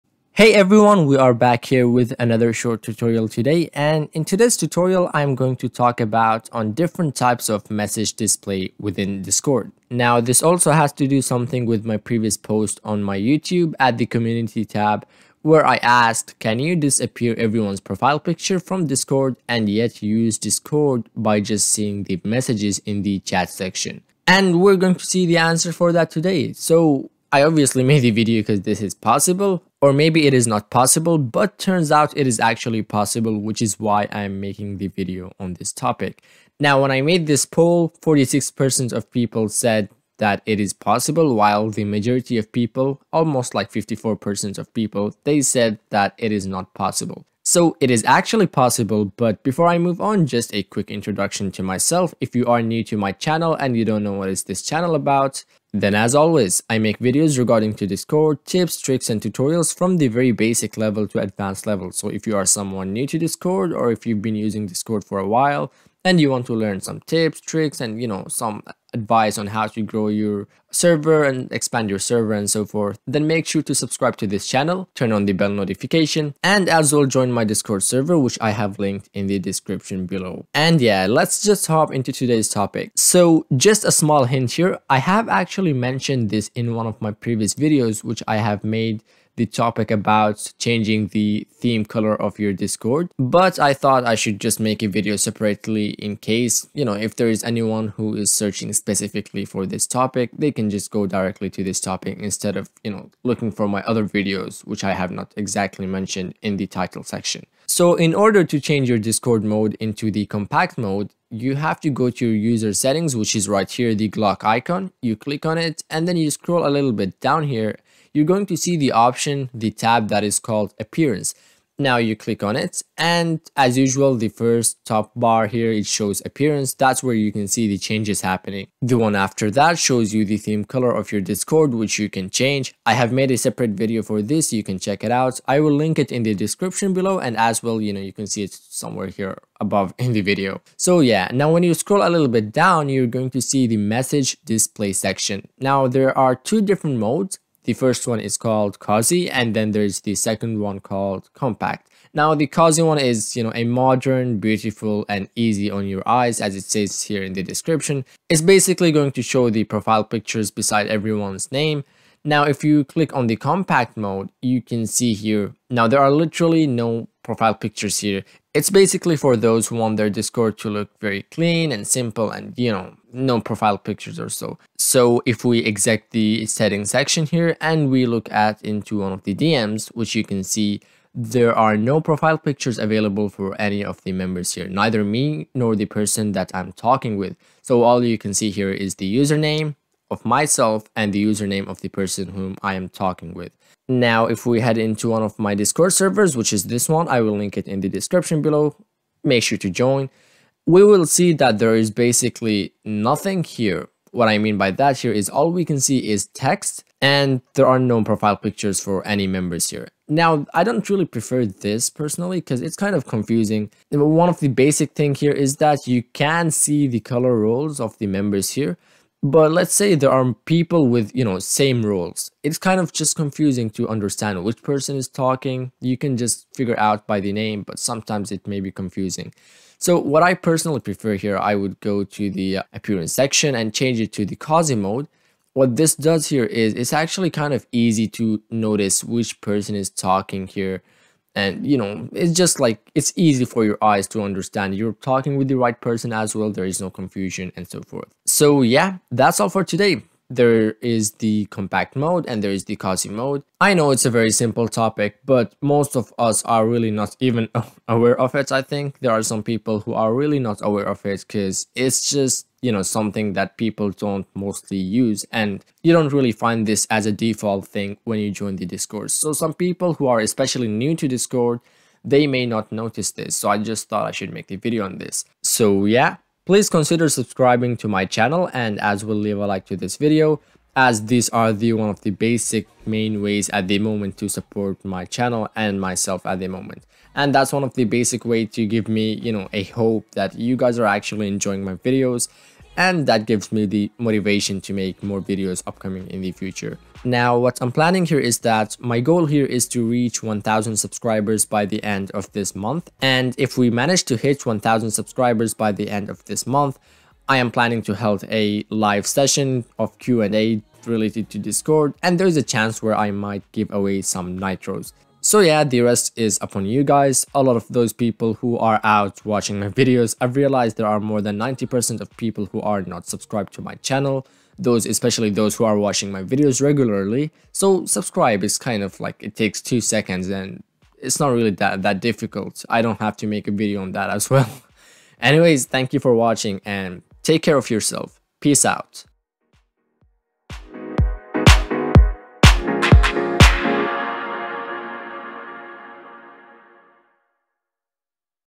Changing Message Display on Discord
Ever heard of Compact mode or Cozy mode for your messages within Discord? Did you know you can hide everyone's profile picture from your screen on Discord? Learn more within this video on how can you change your message displays on Discord!

Join if you're a Nerd and want to gain knowledge and meet some cool people making friends(My other server):